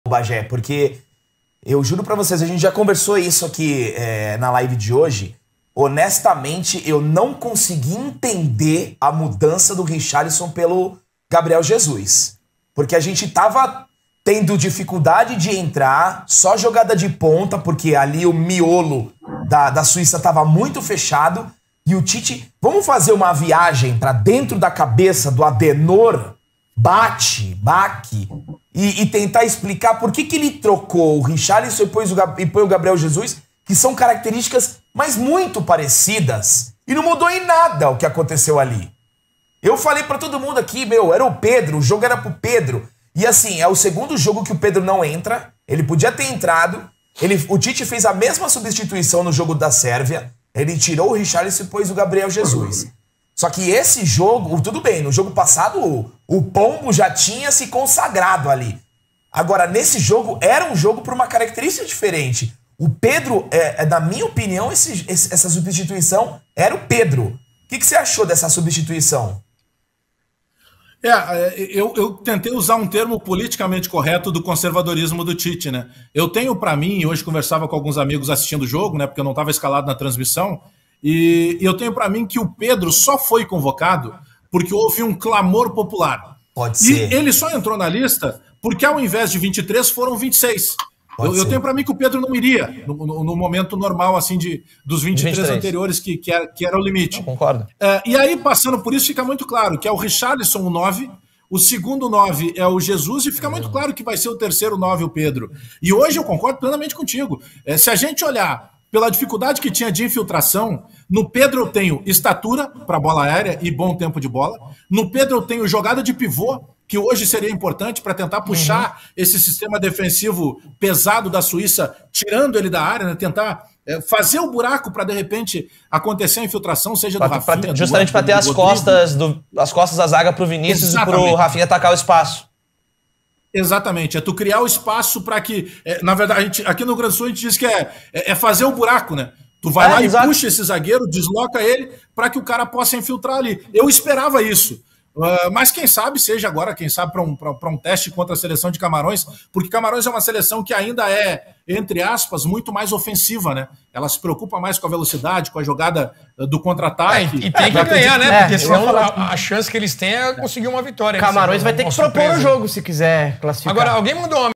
O erro de escalação que Tite cometeu no jogo contra a Suíça
Mesmo com alguns dos melhores atacantes do mundo, a seleção perde muito sem Neymar. O craque é o jogador responsável por criar e armar as jogadas do meio de campo ao ataque adversário. Mas quem colocar no seu lugar? Alex Bagé comenta a escalação de Tite no último jogo e fala sobre Casemiro, Fred e sobre a escalação feita de última hora: “acho que o Pedro só foi convocado por um clamor popular”.

Siga a BrasilParalelo